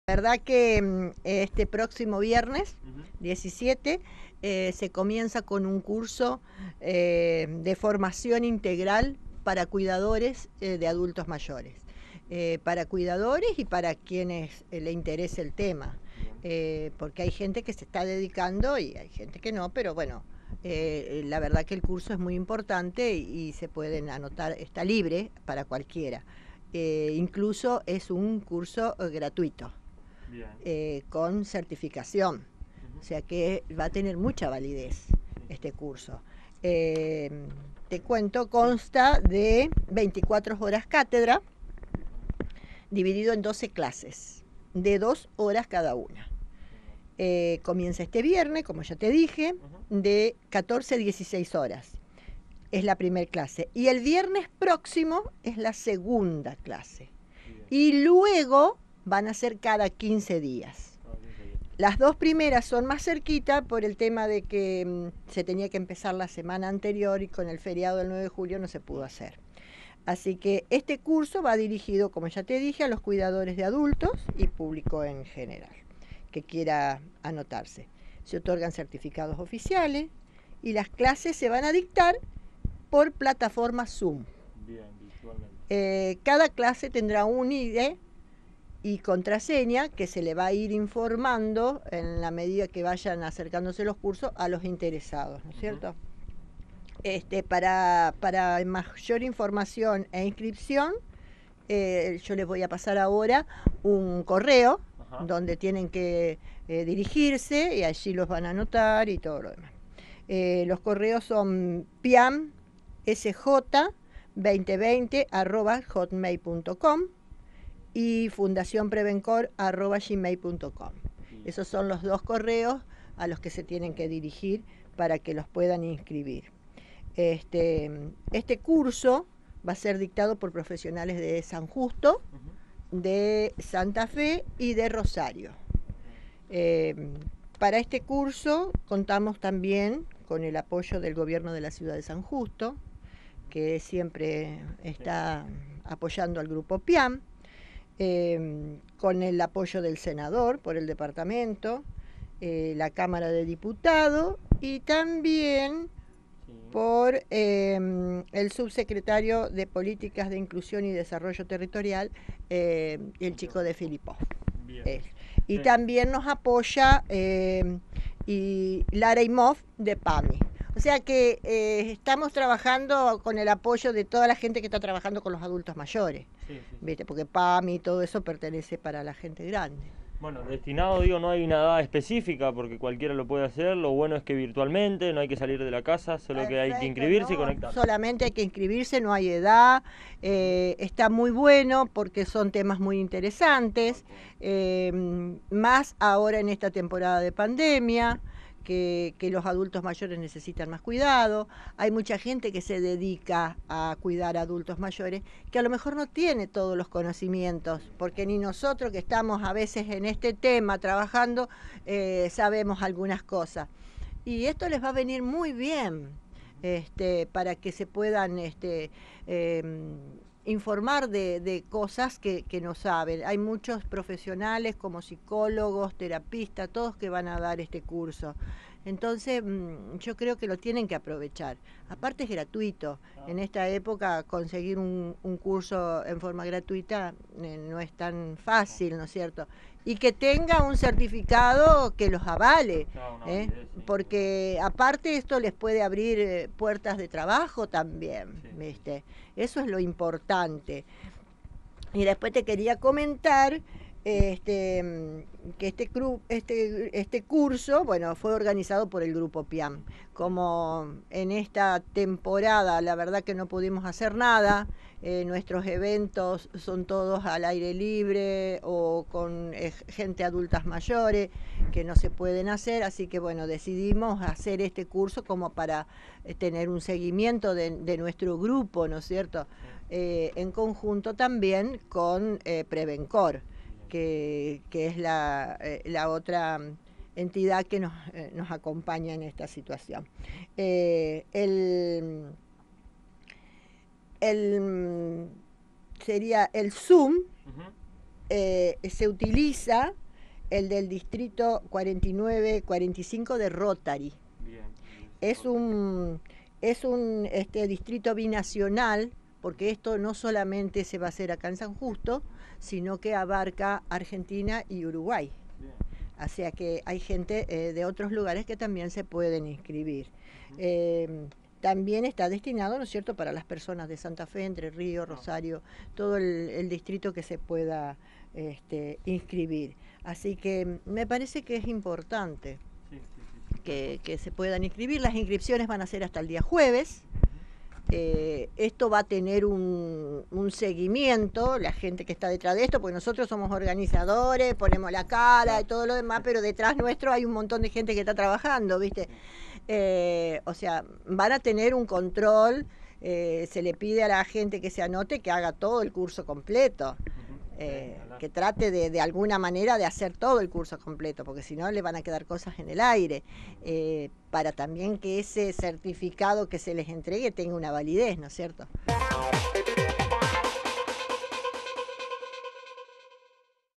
TERESA ARMANDO
Presidente Grupo PYAAM – Realizaran un curso de formación integral de cuidadores de adultos mayores